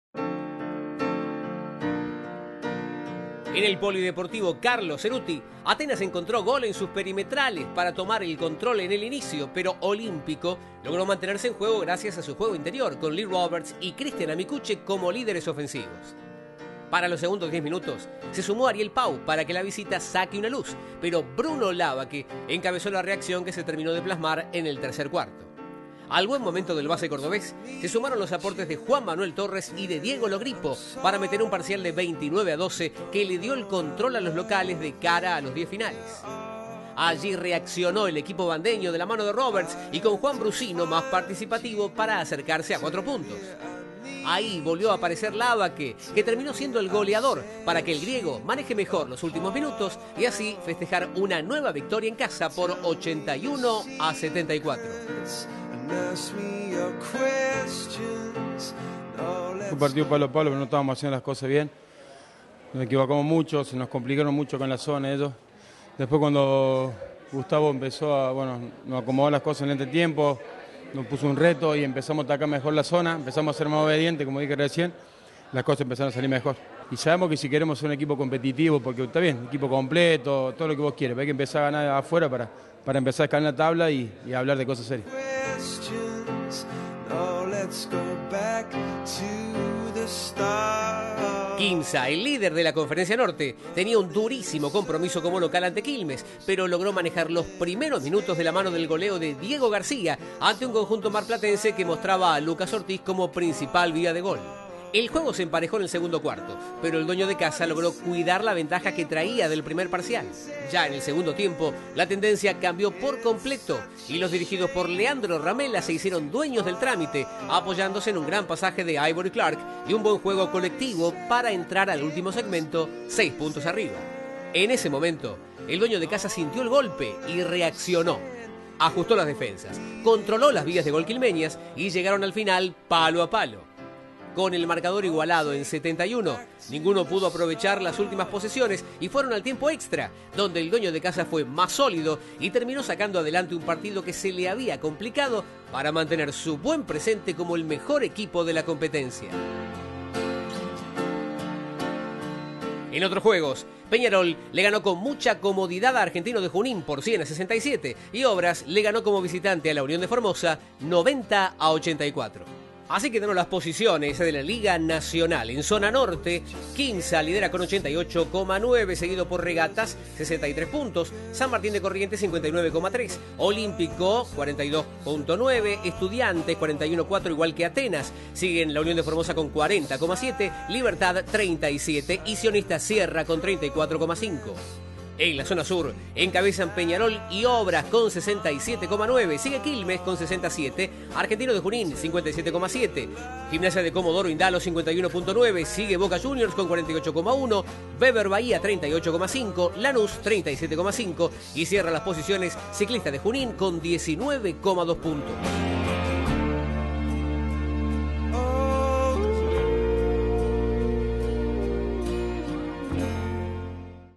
Repaso de Liga (25-1 al 31-1)
Imágenes de los principales juegos de la última semana de Liga nacional de Básquet (25-1-15 al 31-1-15)